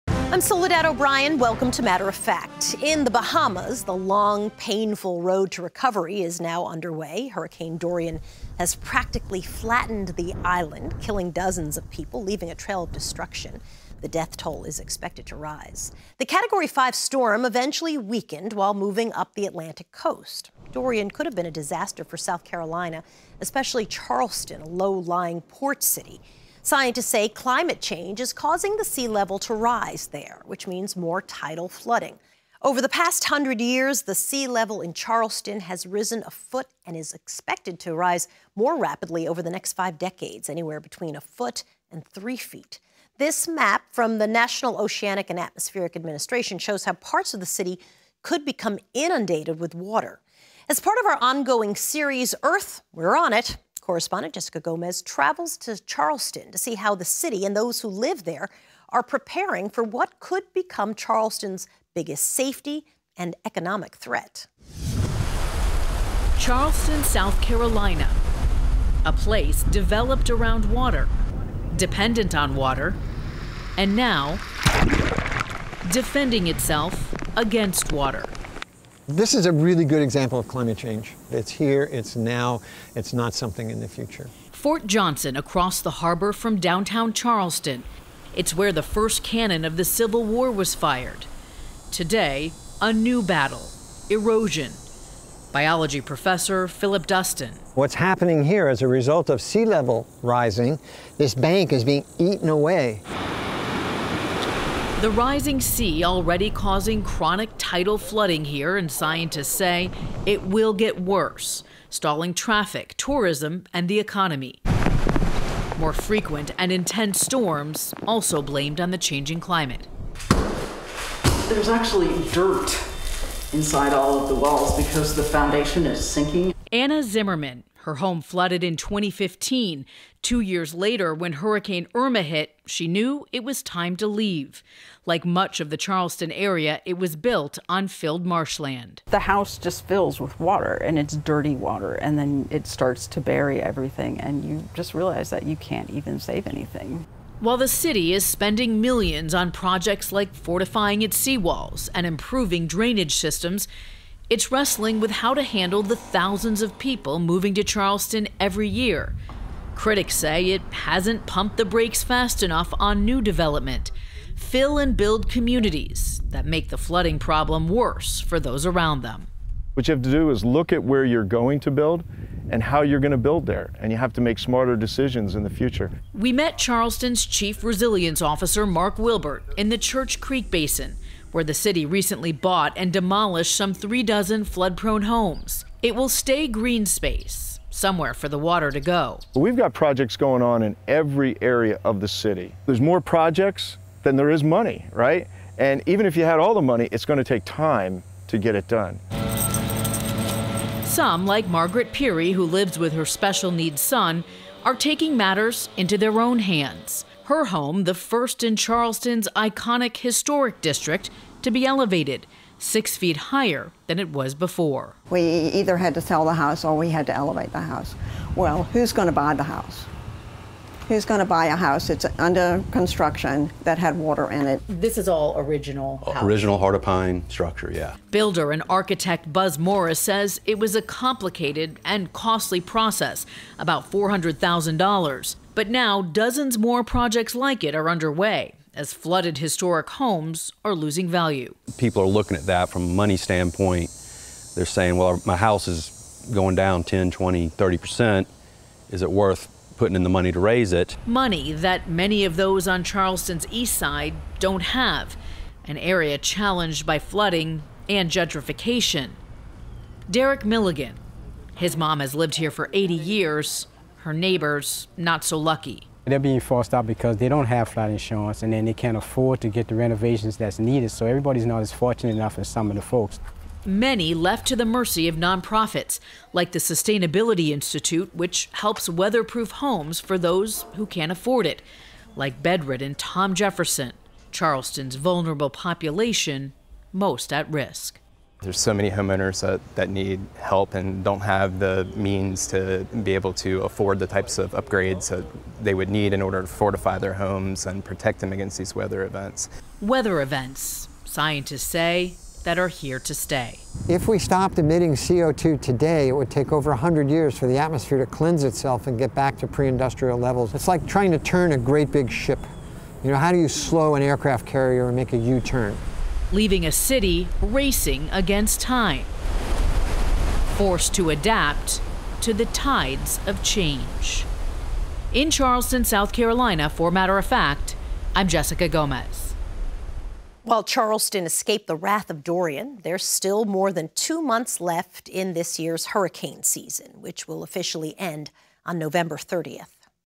Charleston’s Chronic Flooding: How the city plans to survive as sea levels rise
It could become Charleston, South Carolina’s biggest safety and economic threat: the rising sea level. Scientists predict climate change is will cause the sea level to rise anywhere between one to three feet in the next five decades. As part of our series, “Earth, We’re On It,” correspondent Jessica Gomez traveled to Charleston to see how the city and homeowners are preparing for the worst.